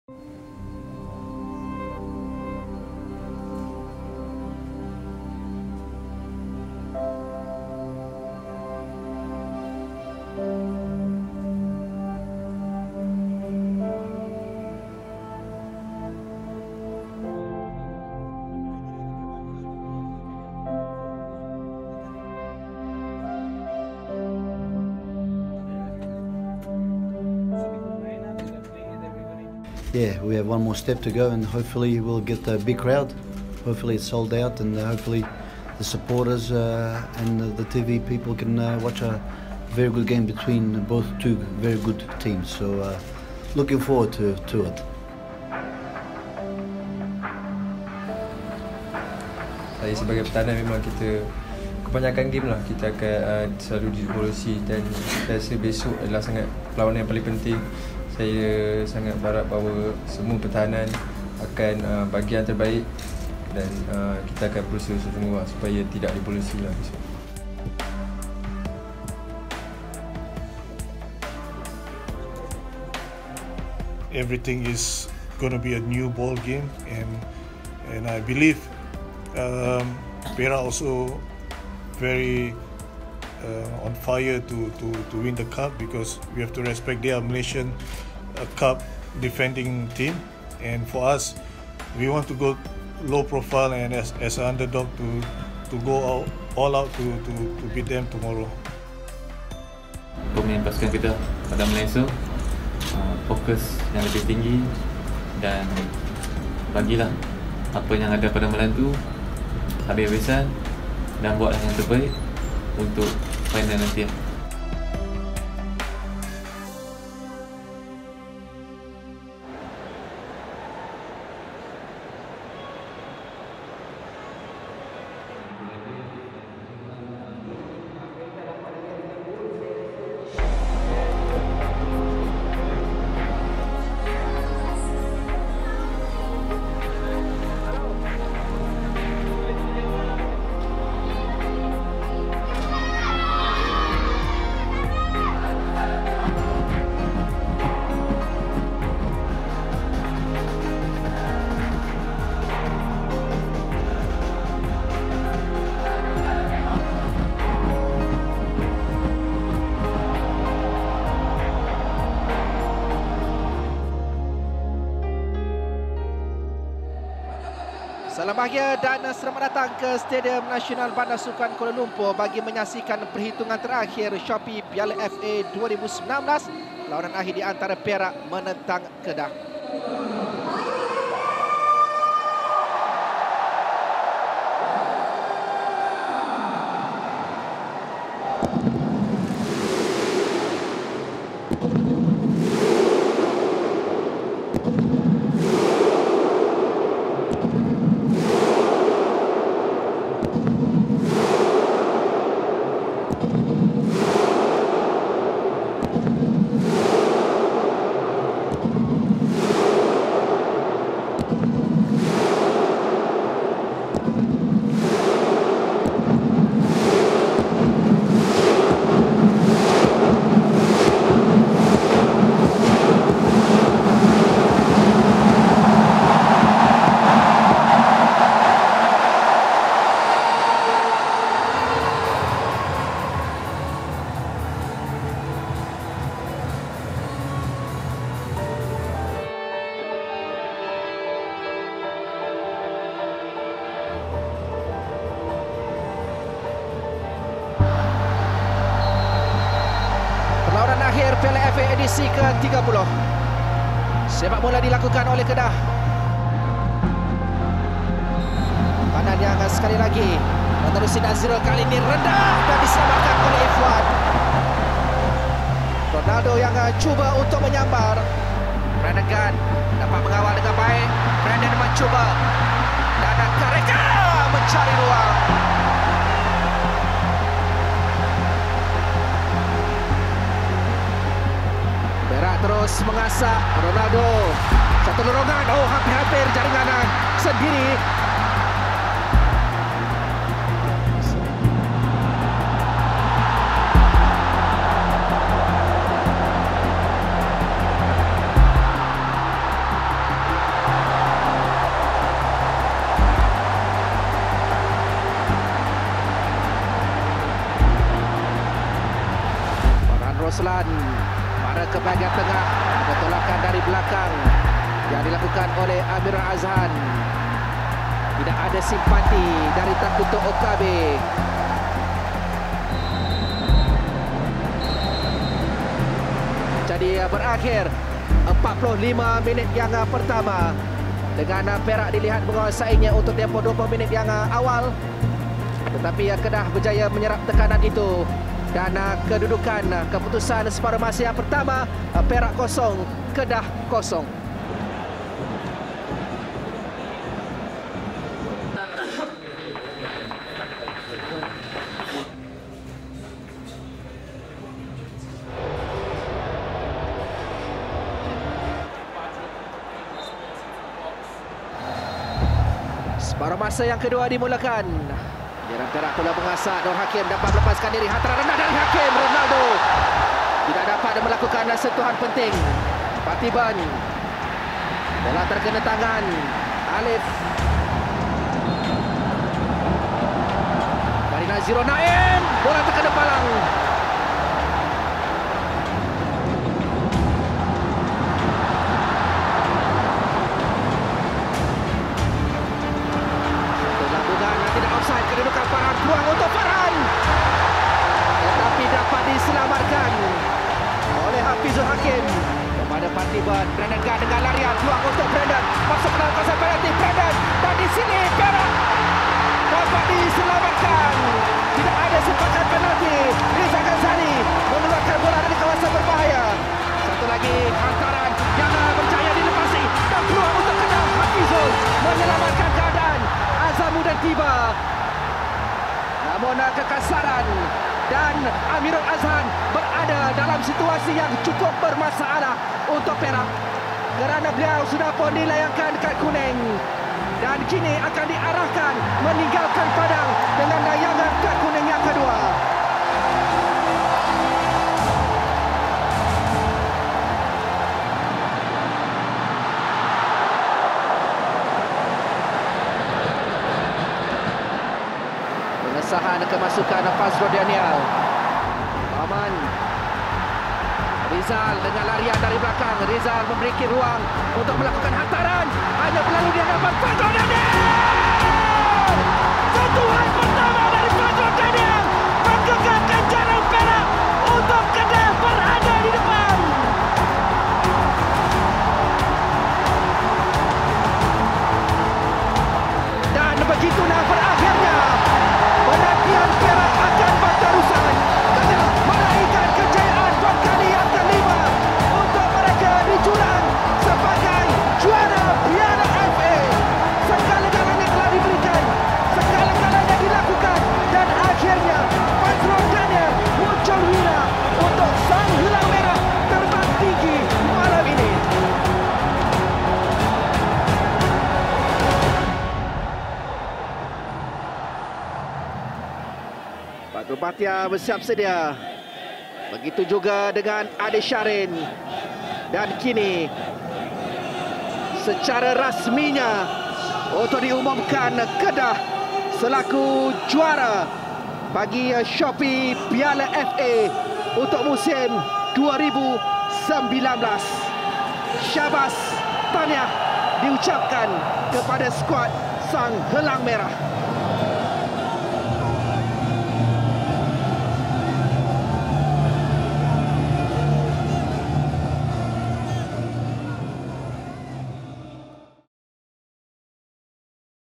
THE INSIDE STORY: Final Shopee Piala FA 2019 | MFL
Saksikan rangkuman perjalanan belakang tabir sehari sebelum dan hari perlawanan  diantara Perak menentang Kedah untuk Final Shopee Piala FA 2019 tempoh hari. Perlawanan berkesudahan dengan jaringan 1-0 oleh Fadzrul Danel pada masa tambahan.